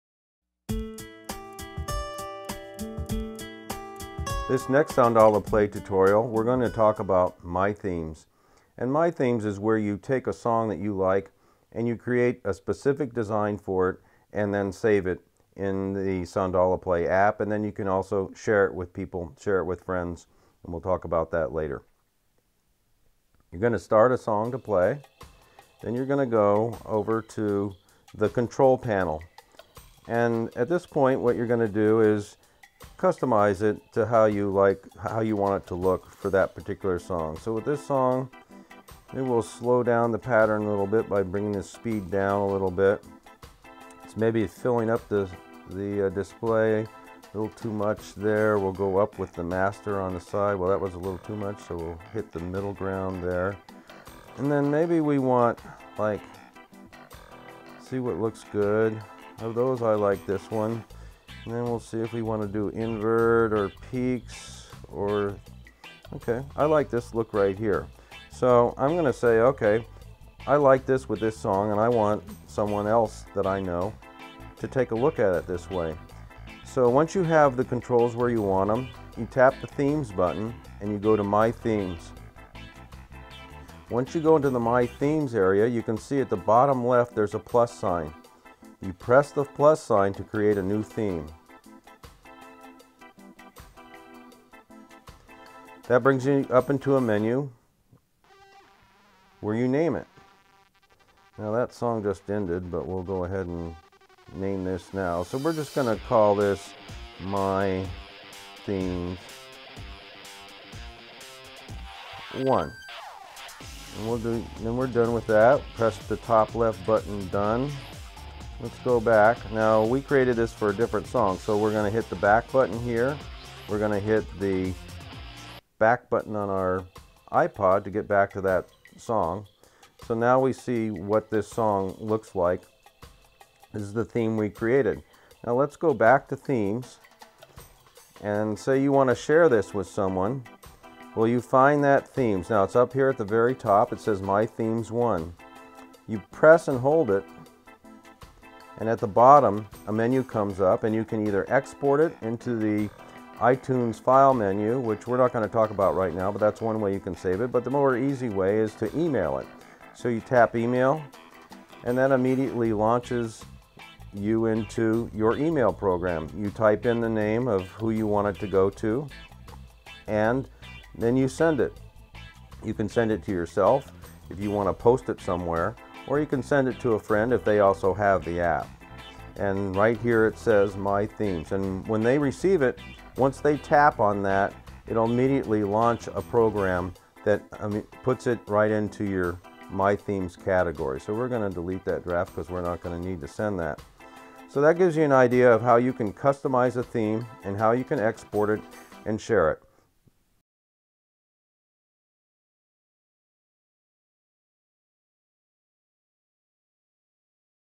Soundala Play Tutorial #6: My Themes
Learn how you can create custom themes based on a particular song with Soundala, the digital music display app available for the Apple iPad. Save your themes and share with friends who also have the app through email or export to your desktop computer.